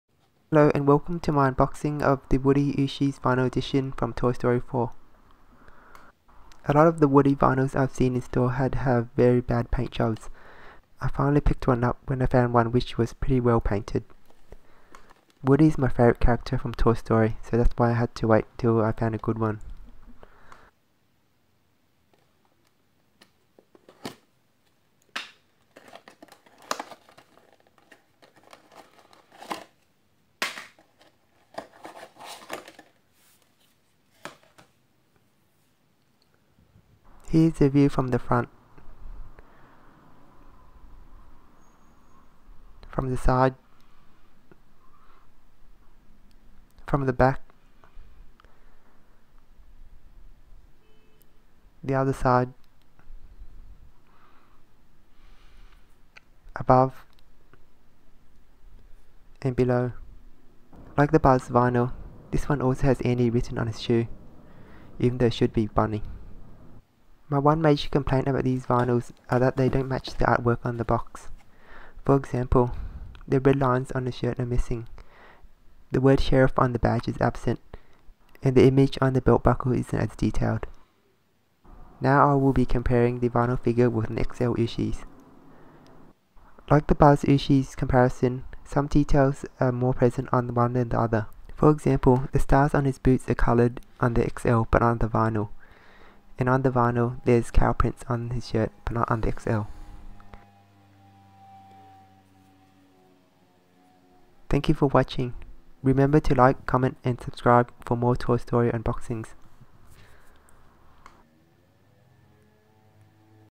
Woody Ooshies Vinyl Unboxing and Review (Toy Story 4)
I open, review and compare the Woody Ooshies Vinyl Edition from Toy Story 4.